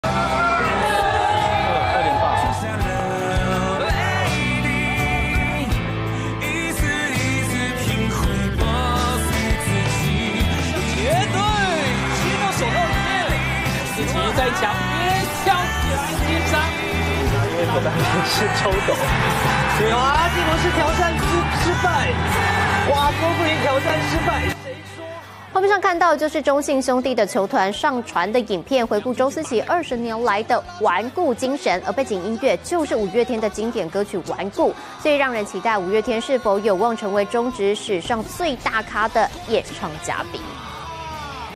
五月天在周思齊引退賽開唱？ 中信球團：不便回應｜鏡新聞
五月天有望搶先江蕙和周杰倫，登上大巨蛋獻唱嗎，中信兄弟球星周思齊9月21日、22日將在大巨蛋，舉行引退儀式，據傳對此，中信兄弟領隊劉志威表示，有好消息一定會告訴大家。